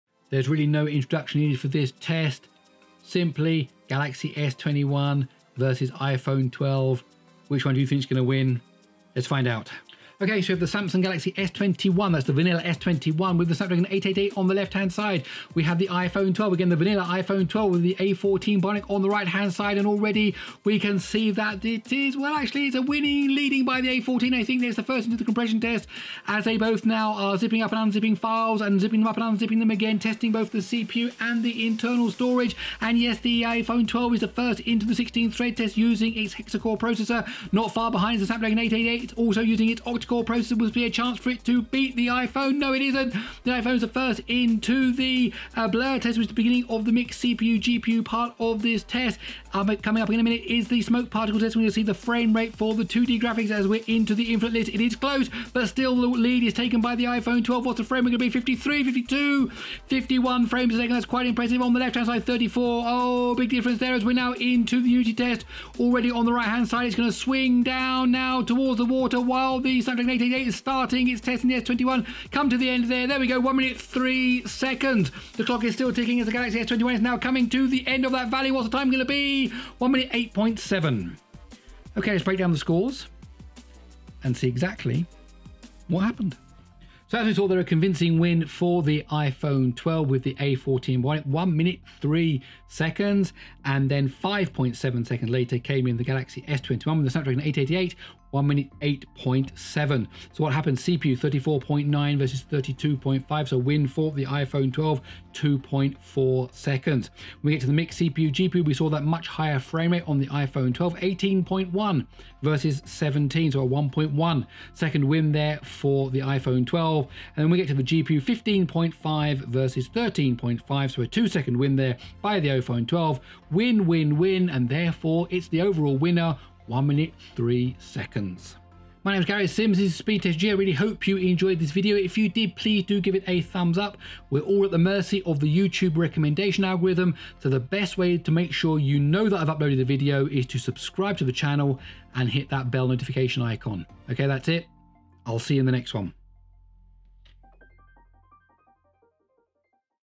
Speed Test: Samsung Galaxy S21 vs iPhone 12
Samsung Galaxy S21 (with the Snapdragon 888) vs iPhone 12 (with the Apple A14 Bionic). No further comment needed!!!